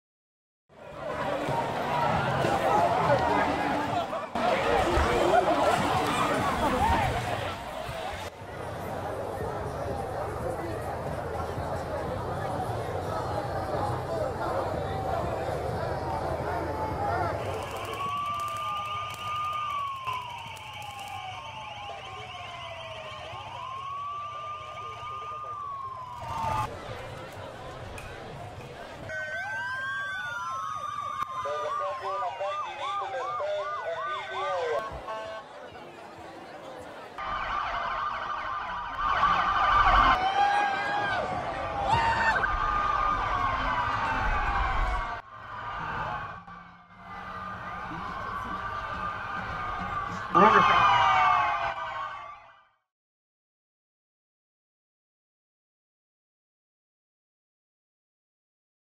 Police crack down on partygoers in Miami defying spring-break curfew
Miami Beach officials have extended a curfew and state of emergency into April, in response to large spring-break crowds of partygoers who have celebrated in the area’s bars and beaches despite the coronavirus. The throngs of young people holidaying in Miami Beach have prompted a crackdown by police, which has included the use of pellets and tear gas fired into the crowds.

More than 1,000 arrests as Miami Beach pushes spring-break curfew into April